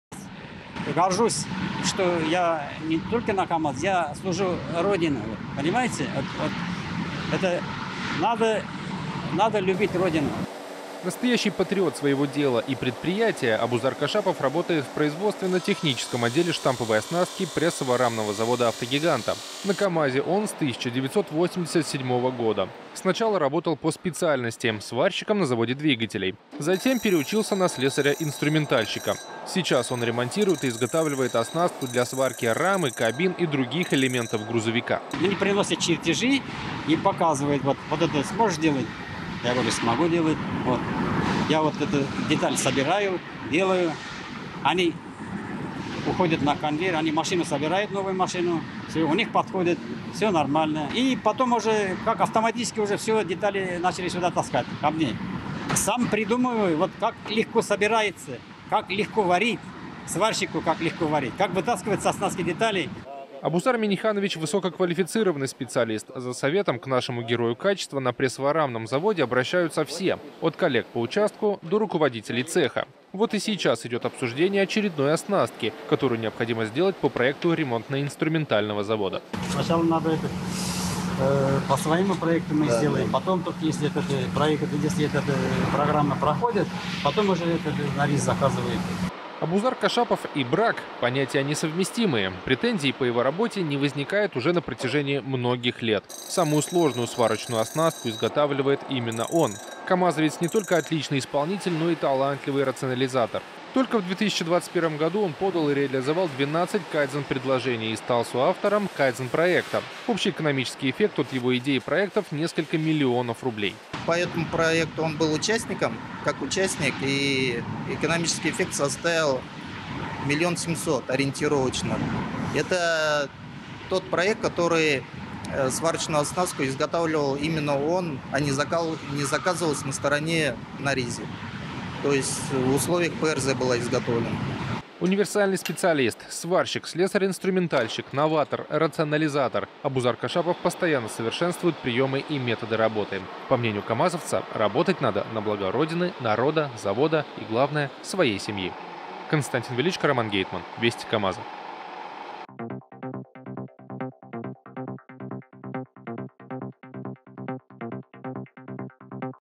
Абузар Кашапов — слесарь-инструментальщик прессово-рамного завода «КАМАЗа»
За что не берётся – всё получается. Подножки, люк, двери, бамперы, крылья передние, крылья задние, стойки, борта. К созданию всего этого и не только приложил руку слесарь-инструментальщик прессово-рамного завода, герой качества Абузар Кашапов. Получив редакционное задание, наши корреспонденты встретились не просто с уникальным камазовцем, но ещё и с настоящим патриотом.

Права на все видеоматериалы принадлежат ПАО «КАМАЗ». Любое их использование возможно только при письменном разрешении правообладателя.